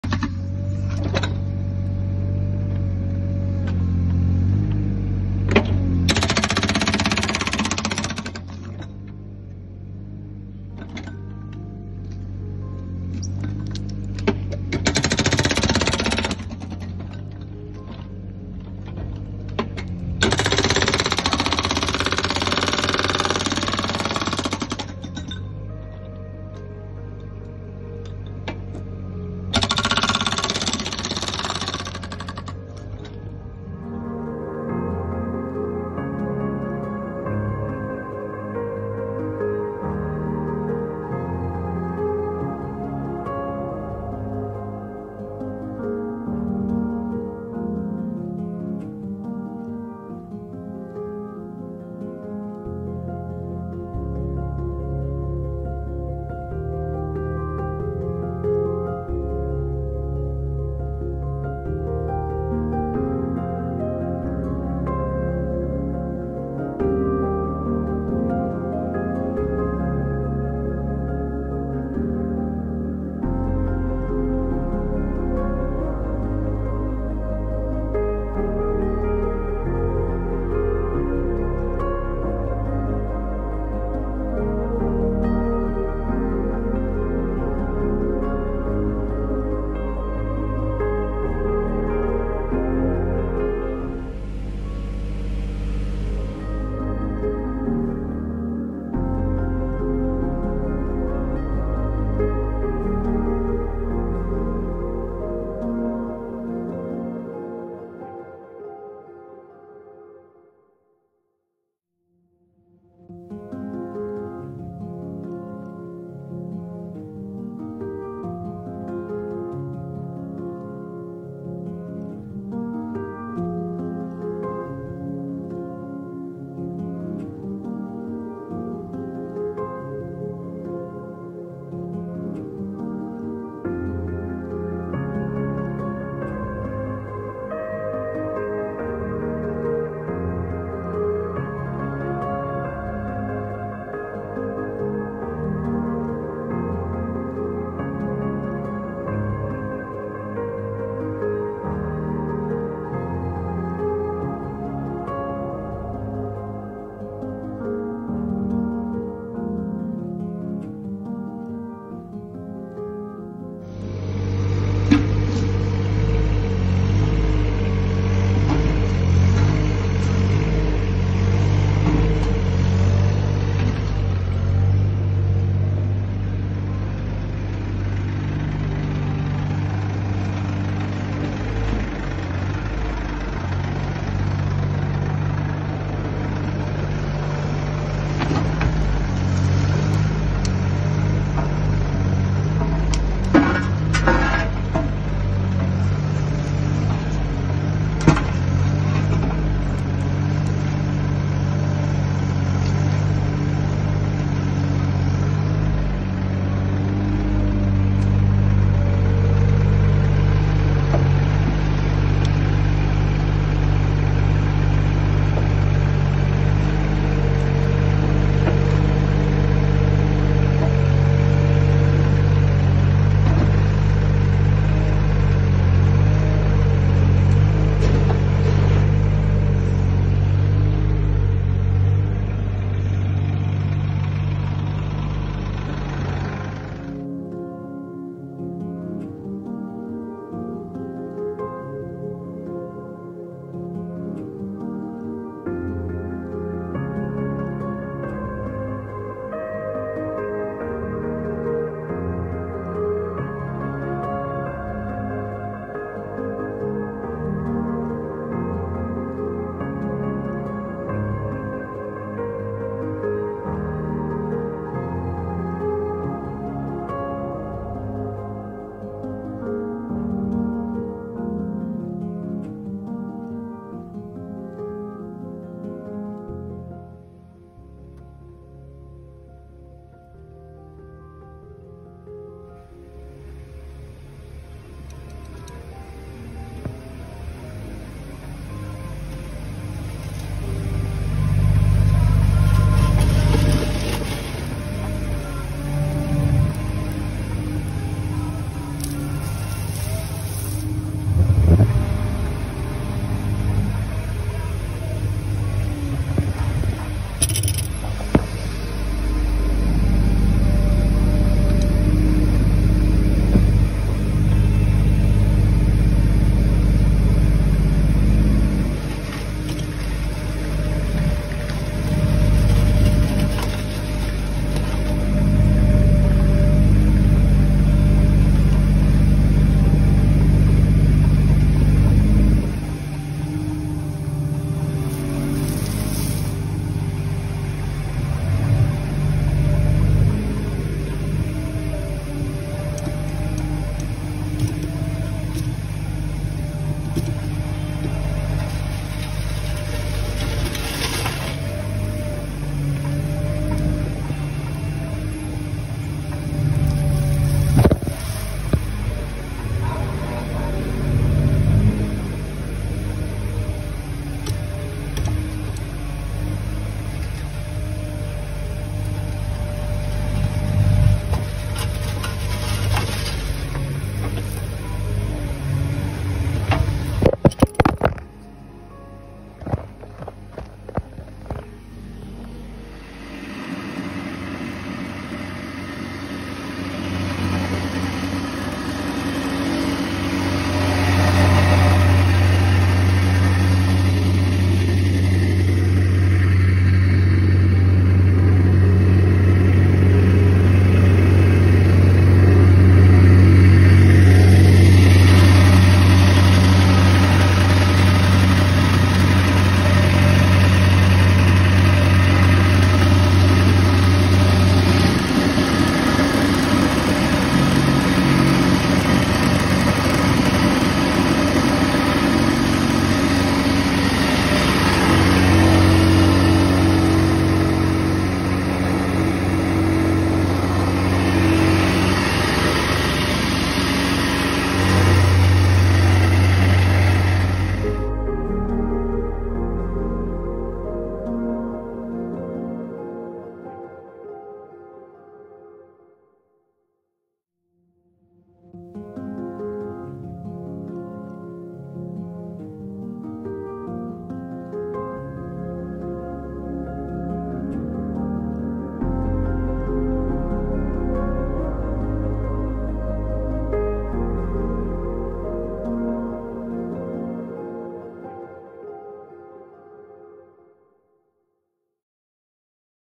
Galabau, Gartenbau, Pflaster, Bagger , Hoti, Hotigartenbau,
Alte Hof Einfahrt
Platten rausgemacht Erde raus
Und neu Pflastersteine verlegt .